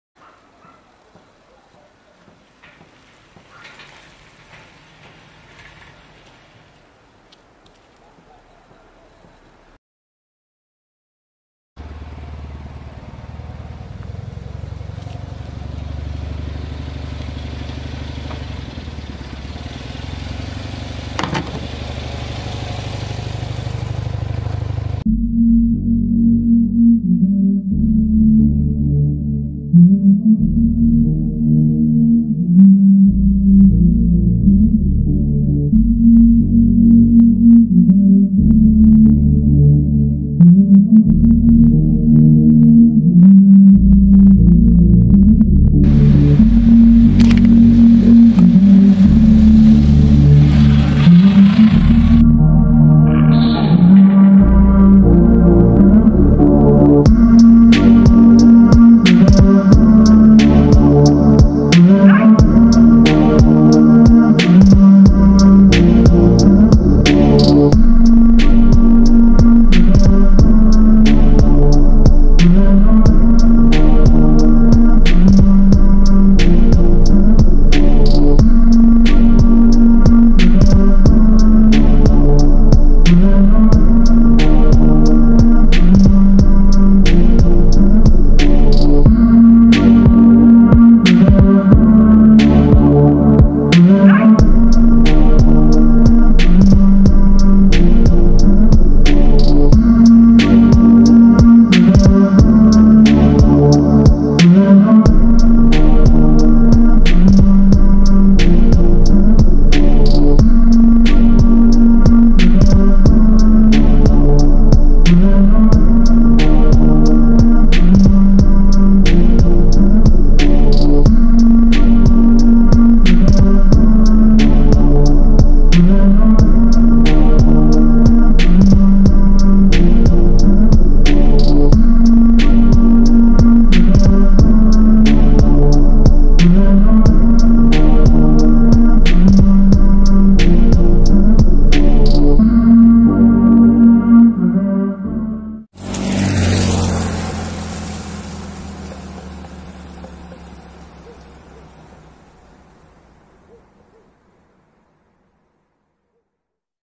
Static Polski Fiat 126p
It's my slammed 1987 Polski Fiat 126p from Hungary.